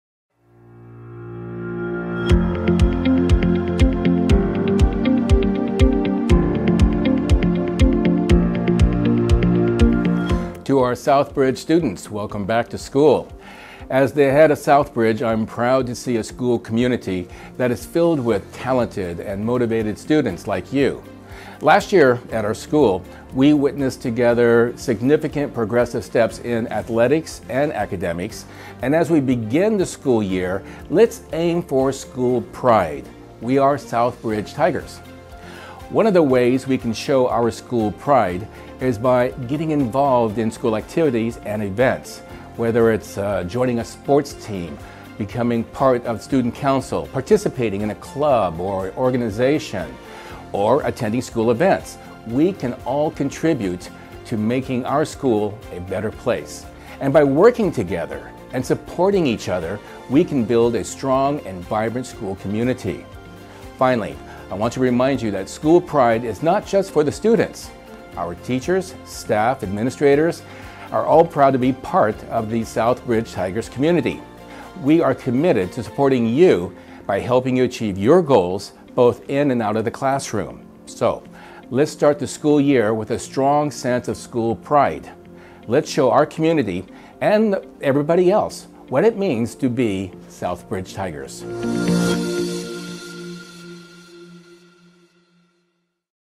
Welcome Back To School
Here is our Head Of School Speech to Welcome New Academic Year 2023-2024 at Southbridge International School Cambodia.